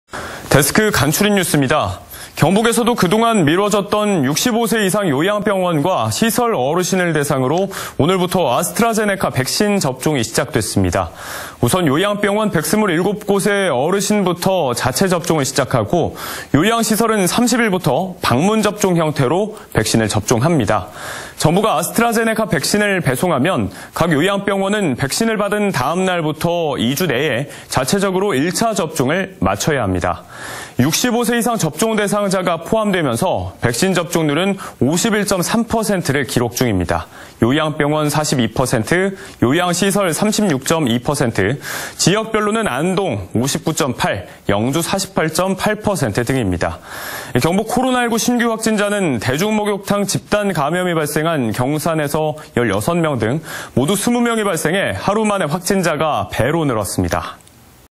65살 이상 요양병원부터 백신 접종 시작 / 안동MBC
2021/03/23 10:24:28    작성자 : 이정희

경북에서도 그동안 미뤄졌던 65세 이상
요양병원과 시설 어르신을 대상으로 오늘부터 아스트라제네카 백신 접종이 시작됐습니다.
◀ＥＮＤ▶

우선 요양병원 127곳의 어르신부터 자체 접종을 시작하고, 요양시설은 30일부터 방문접종
형태로 백신을 접종합니다.

현재까지 경북 백신 접종률은
51.3%를 기록 중입니다.

한편 경북 코로나19 신규 확진자는
대중목욕탕 집단감염이 발생한 경산에서
16명 등 모두 20명이 발생해
하루 만에 확진자가 배로 늘었습니다.